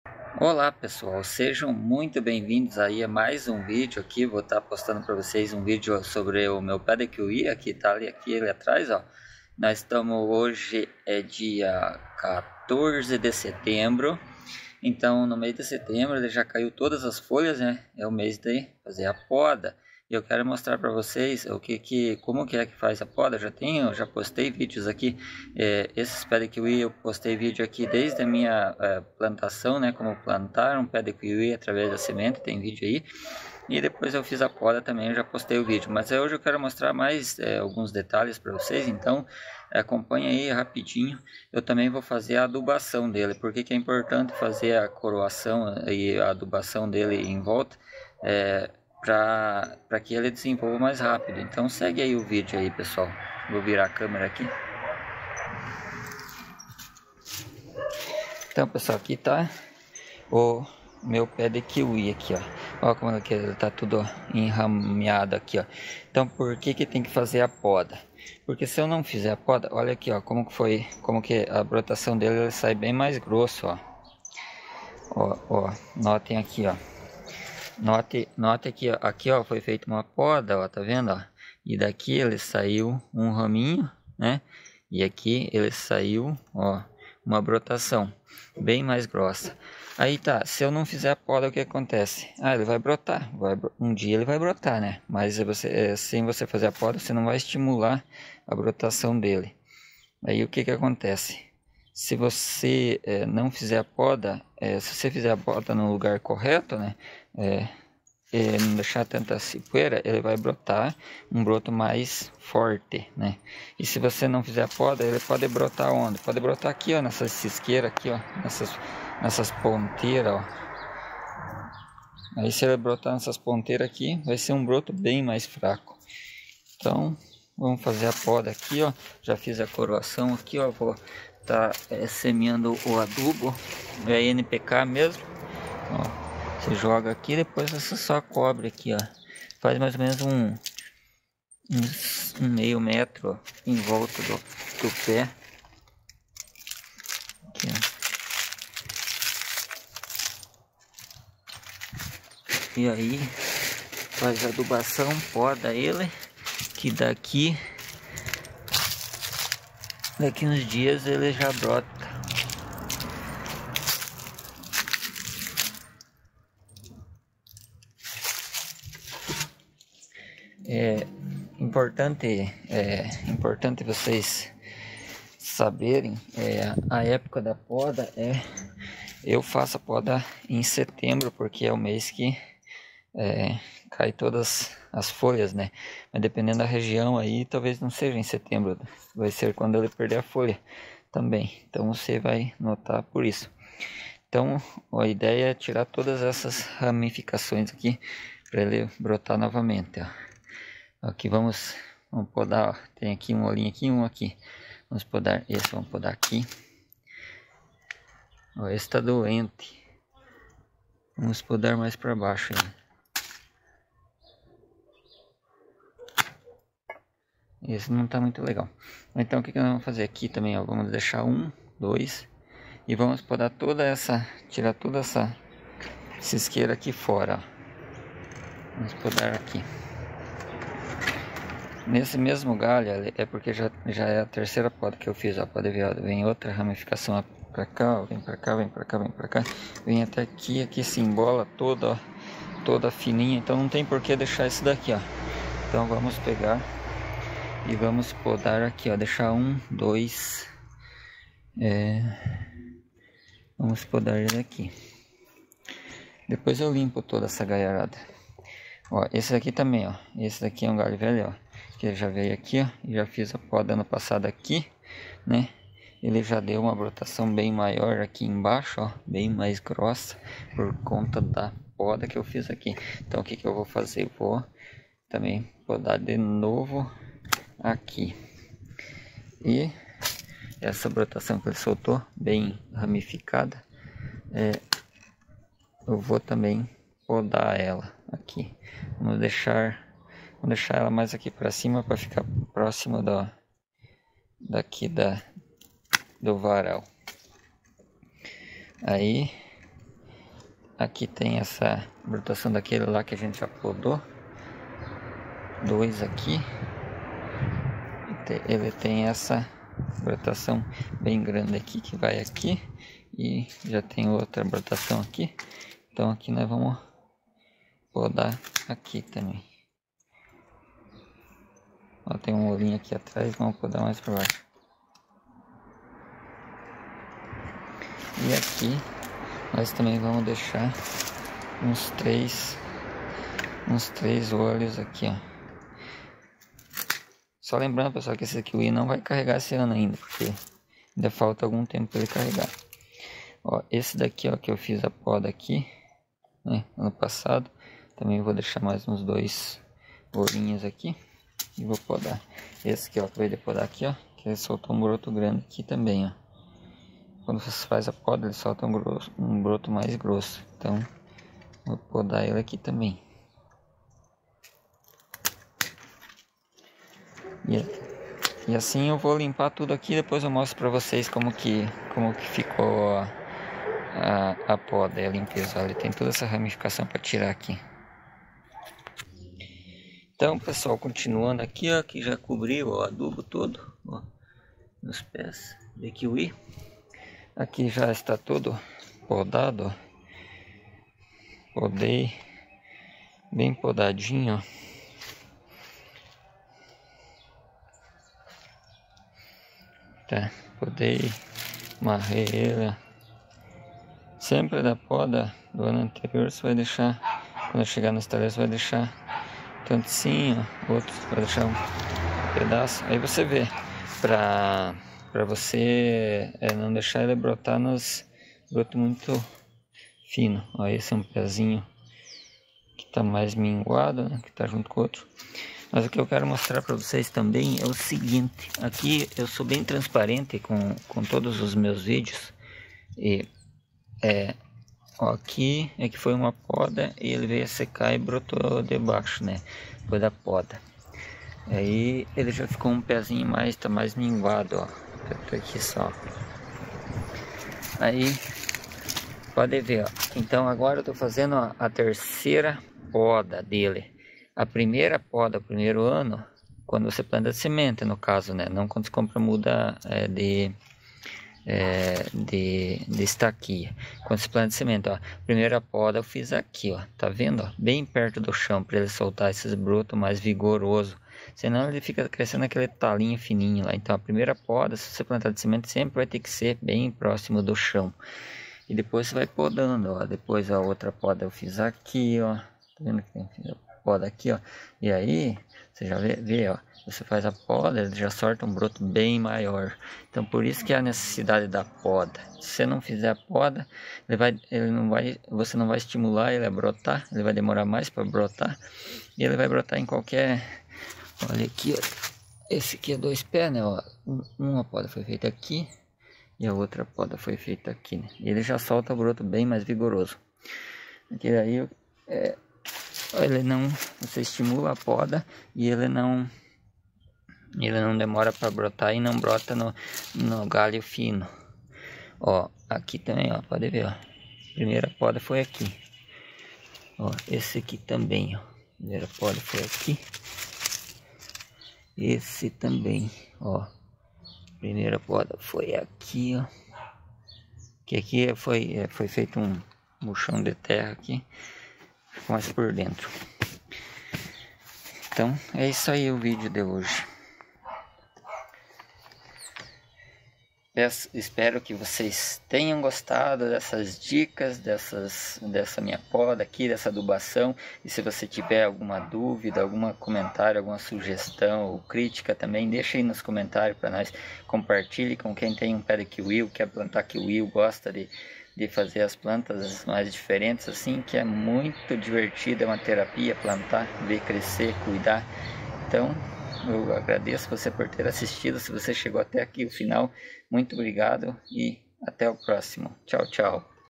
Adubação e dicas de poda do Kiwi
Nesse vídeo eu faço a adubação correta e faço a poda do kiwi. Dou dicas de como fazer adubação do kiwi, também dicas de como fazer uma poda de formação correta, a importancia da poda do pé de kiwi